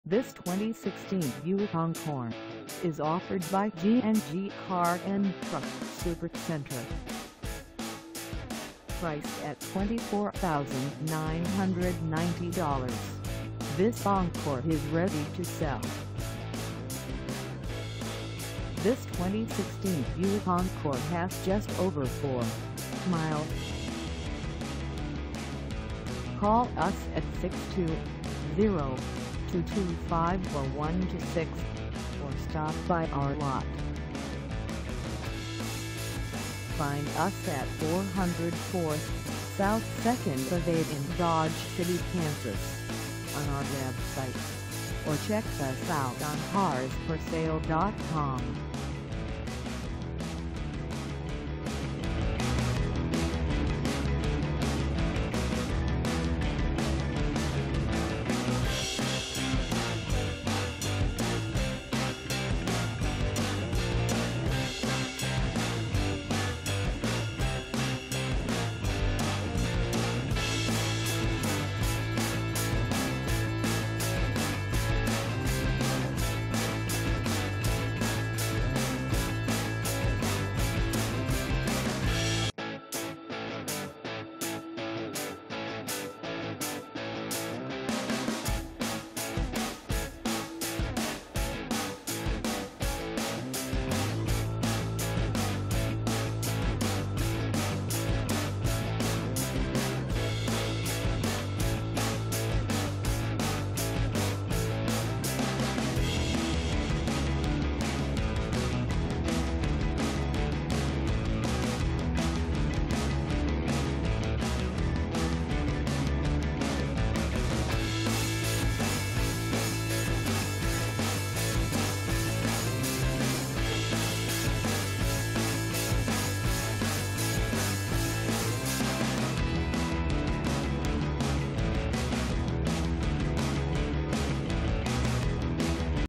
2016 Buick Encore Base 4dr Crossover for sale in Dodge City,
This 2016 Buick Encore Base 4dr Crossover is for sale in Dodge City, KS 67801 at GandG Car and Truck Supercenter.

Engine: 1.4L I4
Transmission: Automatic 6-Speed
Color: White
Mileage: 4
Contact GandG Car and Truck Supercenter 404 S 2nd Ave Dodge City, KS 67801 to test drive this 2016 Buick Encore Base 4dr Crossover today.